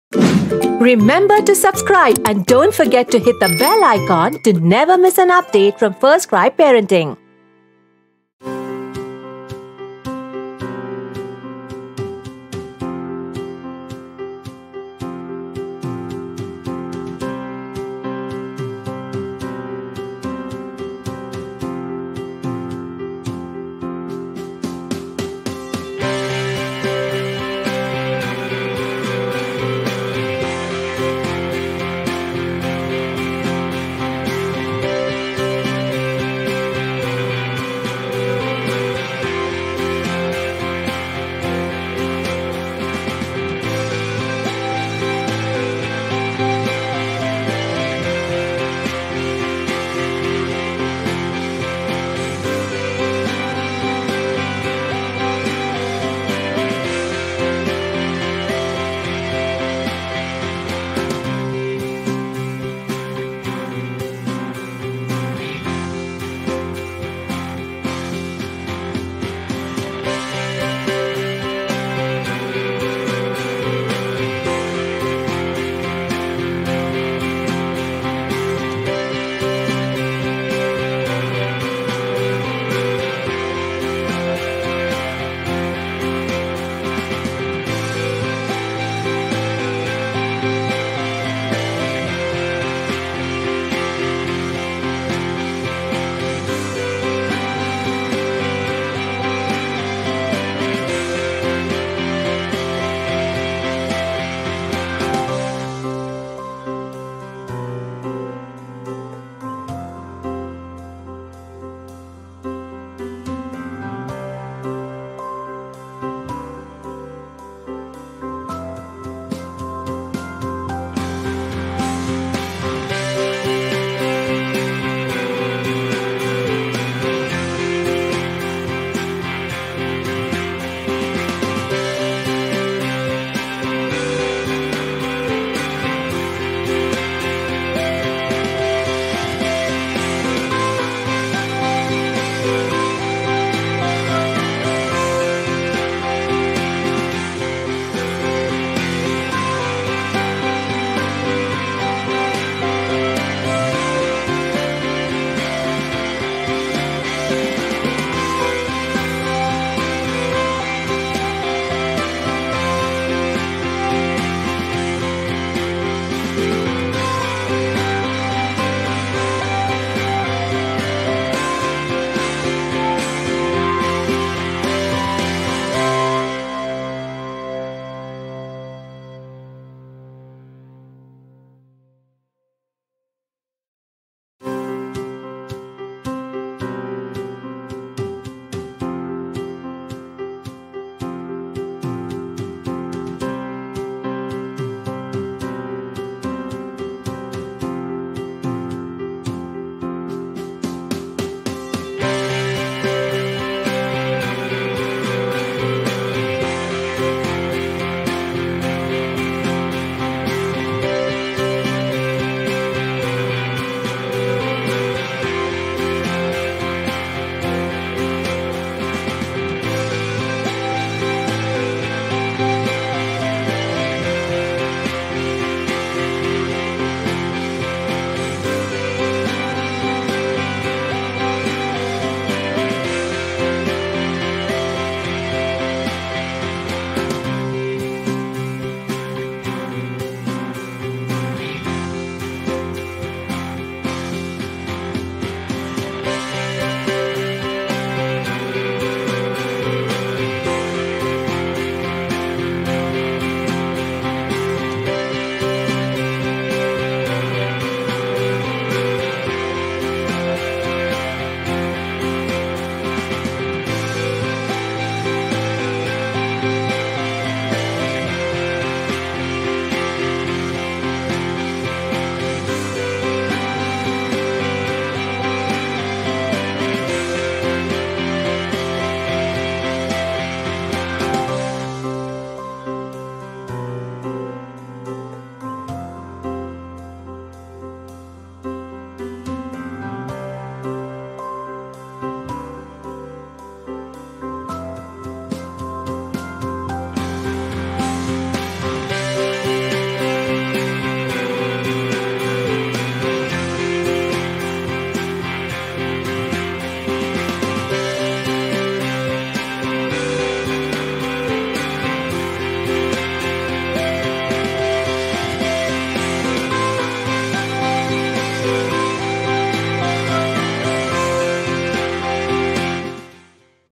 10 Simple and Healthy Finger Food Ideas for Toddlers
Toddlers are fussy eaters. If you want to make them eat, finger food is one of the best ways to go. This video provides you with some simple and healthy finger food ideas that will make it to feed your active toddler easier. Your child will surely love to eat these foods on his own.

Feeding toddlers is challenging. They either busy in some activity or want to eat on their own at this age. Moreover, as they are in a growing phase they don’t eat a full-size meal. Thus it becomes important to feed them frequently in small amounts. Finger foods make it easier for you to feed your toddler all day long while ensuring he gets much-needed nutrients for his development. Watch this video to know some easy and healthy finger food ideas for your toddler.